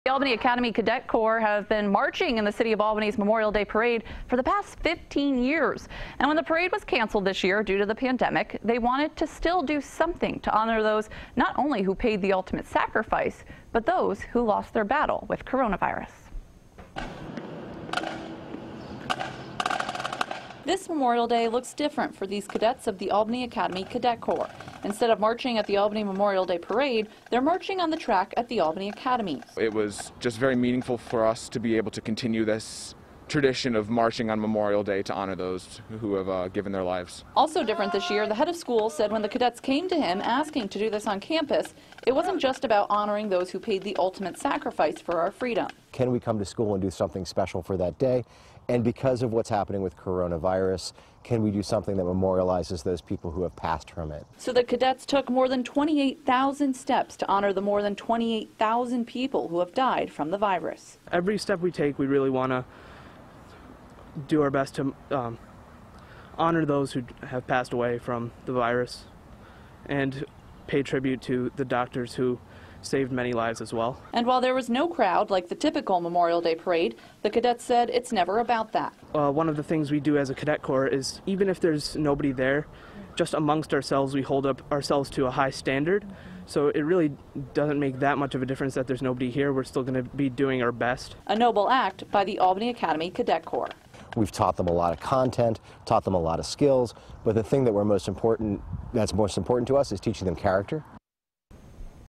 Albany Academy cadets find unique way to continue Memorial Day tradition
The Albany Academy Cadet Corps have been marching in the city of Albany's Memorial Day parade for the past 15 years. When the parade was canceled, they wanted to still do something to honor not only those who paid the ultimate sacrifice, but those who lost their battle with coronavirus.